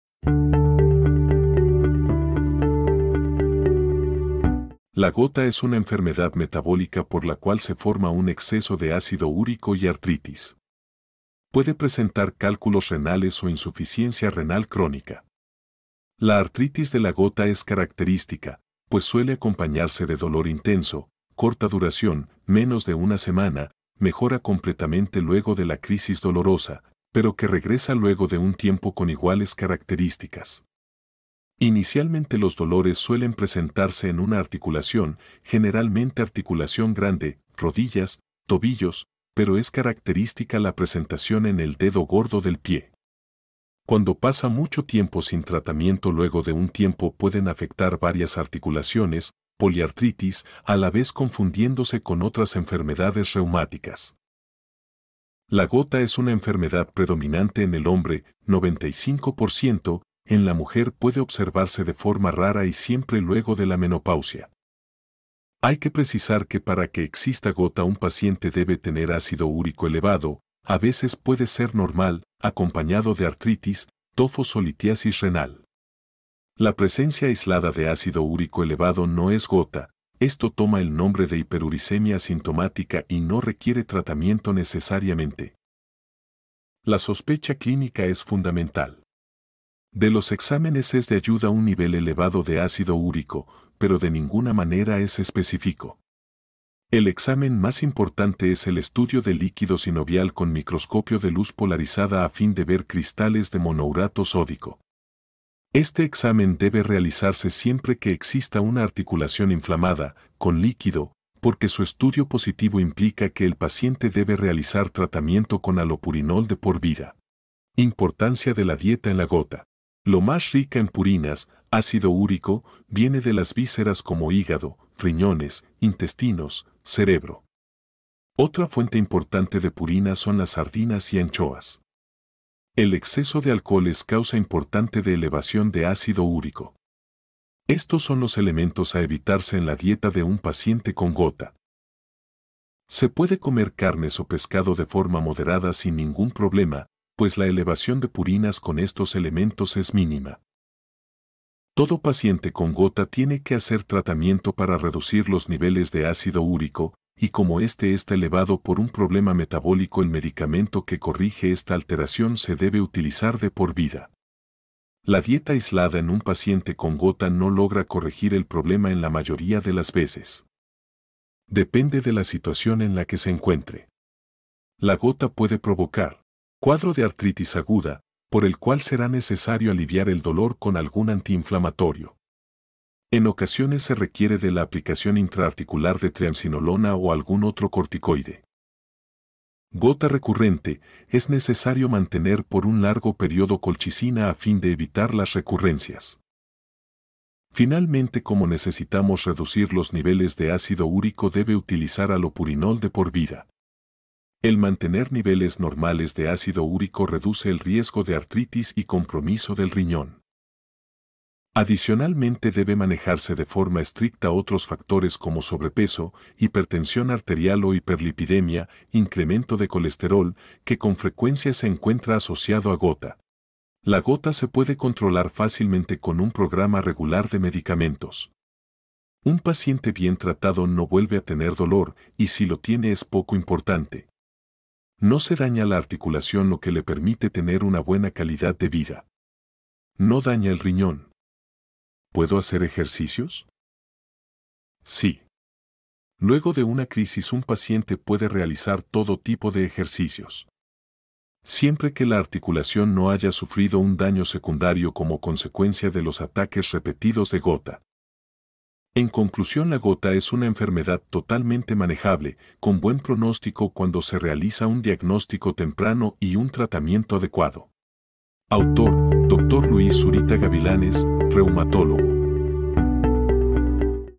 La Gota - Enfermedad Reumática - Ácido Úrico
En este video hablaremos sobre la gota. La gota es una enfermedad reumática que debe ser diagnósticada a tiempo para evitar complicaciones.
Dr. Luis Zurita G
Reumatólogo Guayaquil